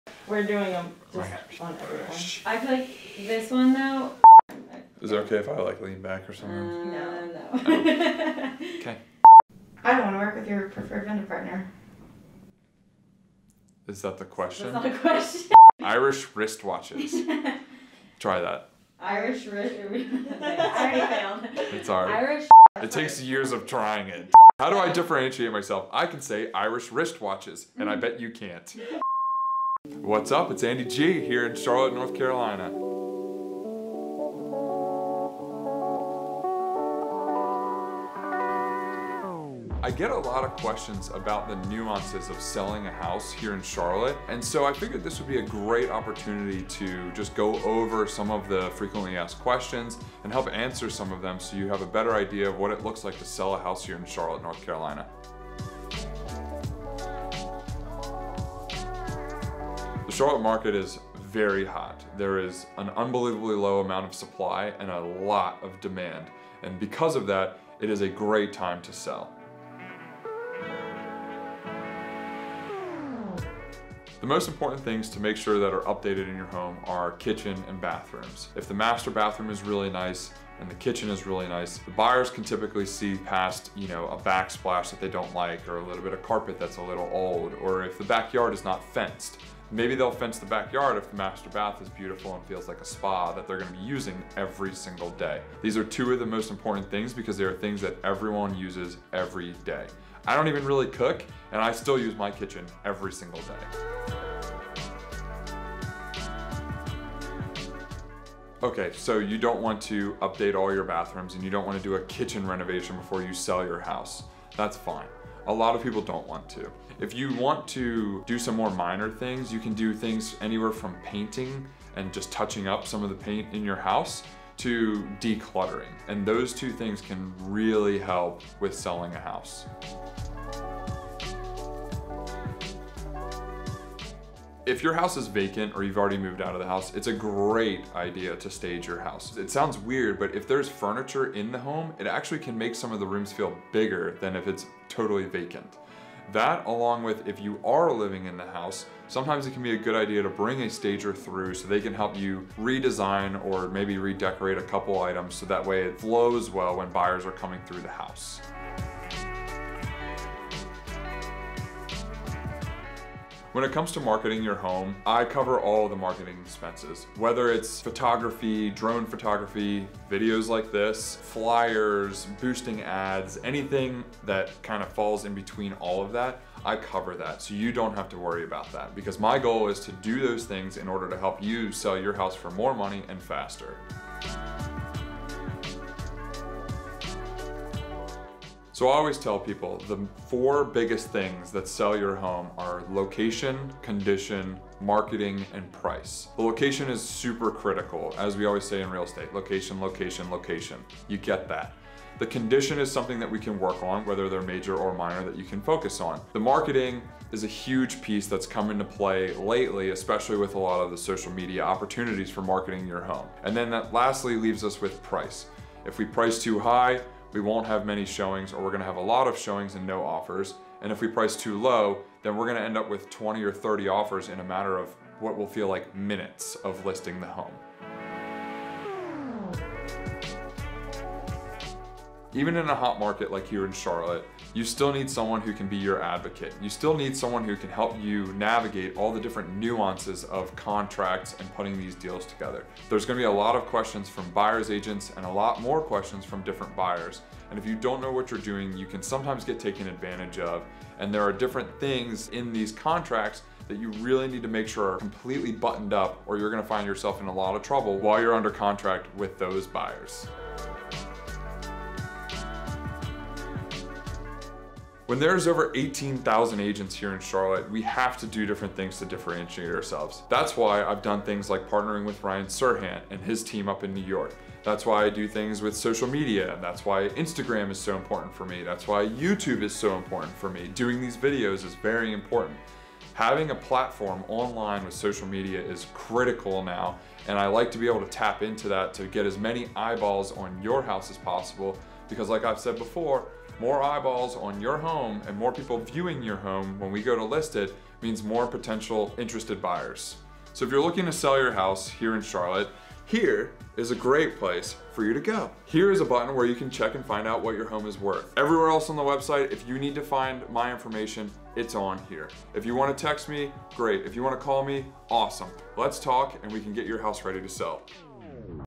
Things you NEED to know if YOU plan to SELL in Charlotte, NC
🏡Thinking about selling your home in Charlotte, NC? I'm sure it comes as no surprise to you that this is one of the hottest markets in America. There are so many little details that go into properly selling a home, even in. a scorching hot🔥market. Do you have so many unanswered questions you don't even know where to begin? Watch this video and you'll be able to get a good understanding of some of the major things to think about when you're preparing to sell your home here in Charlotte.